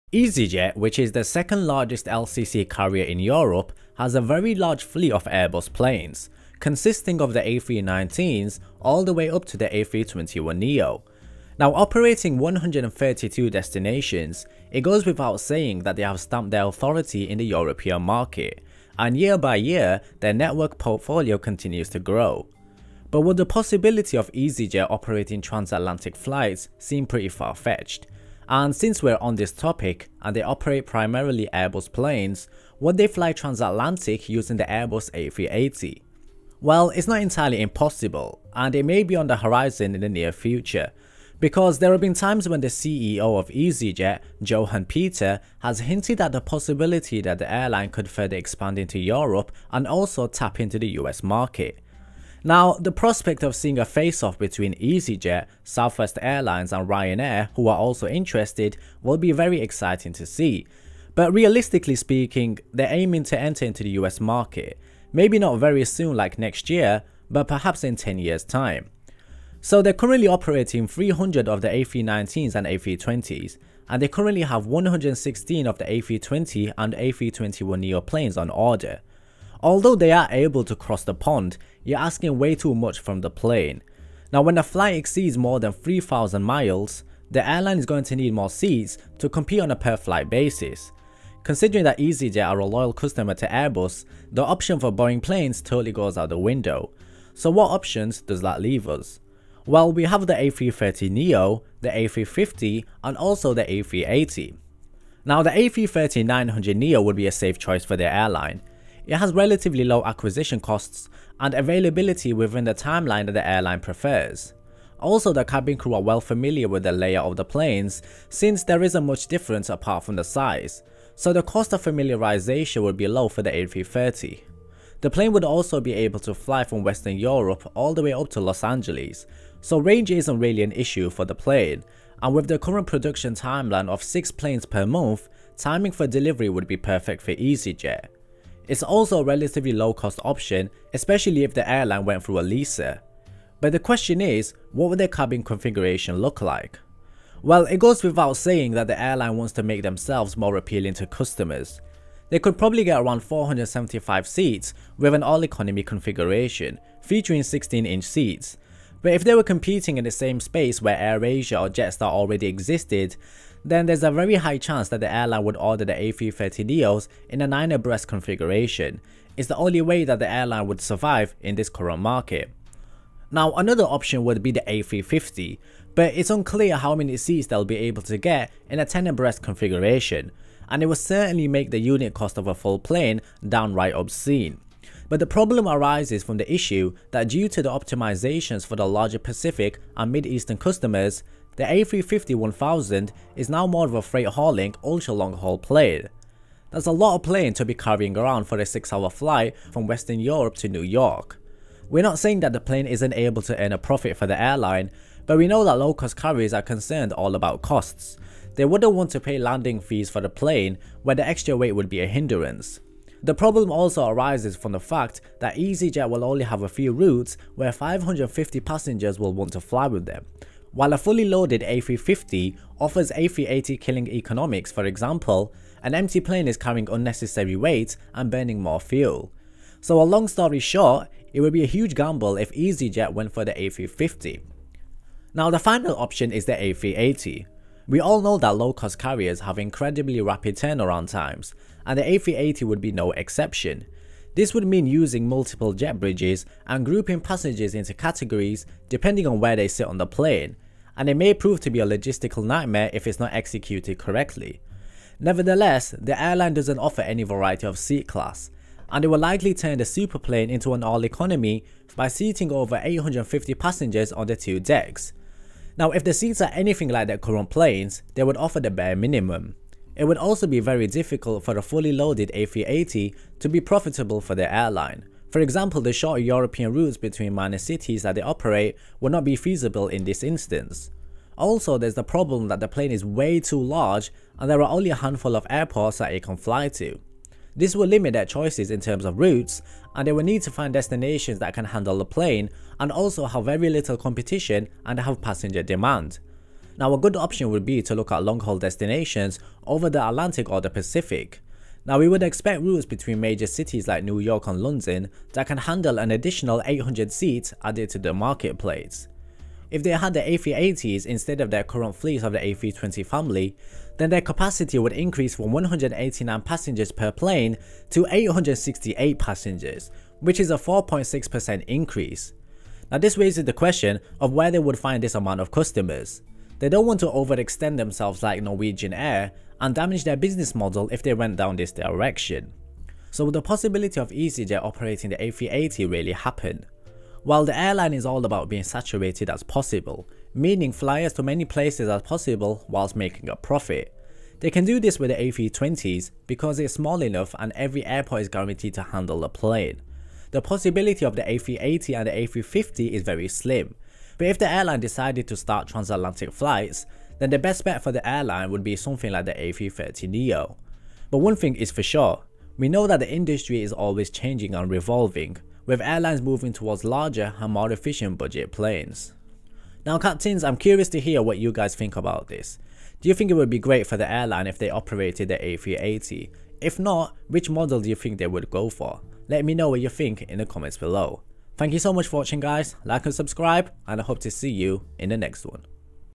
Could EasyJet Fly The Airbus A380?
Easyjet is the second largest LCC carrier in Europe. Johan Peter, CEO, has hinted at the possibility that the airline could expand into the North American market. In this video we'll discuss their plane possibilities and which aircraft could suit their needs.

Navgeek Aviation